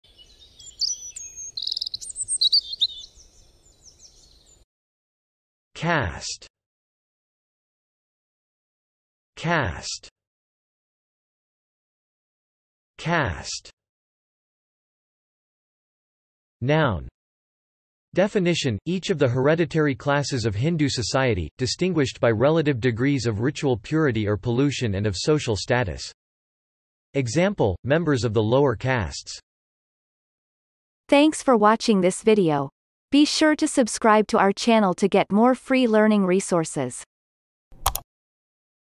How to pronounce caste - Vocab Today
The pronunciation of the word caste in Amercian accent is demonstrated in this video. If the word is from another language, such as brand name, it will be pronounced in its original language. Based on the word frequency, this is of 3 stars difficulty.

The word caste has the following meanings:
1. (noun)
Definition : Each of the hereditary classes of Hindu society, distinguished by relative degrees of ritual purity or pollution and of social status.
Example : members of the lower castes